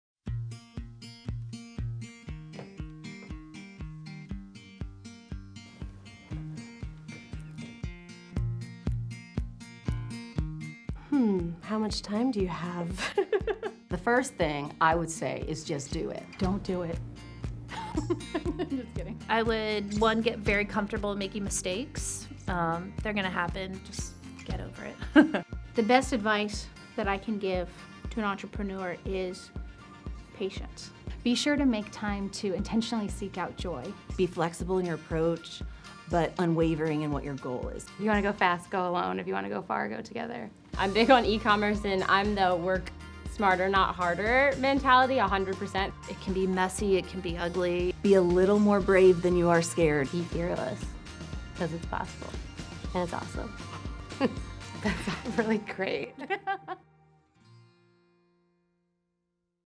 The Powerful Women of eBay
For International Women’s Day we sat down with successful female business owners and asked them to share their advice with aspiring entrepreneurs. Their responses were very inspiring…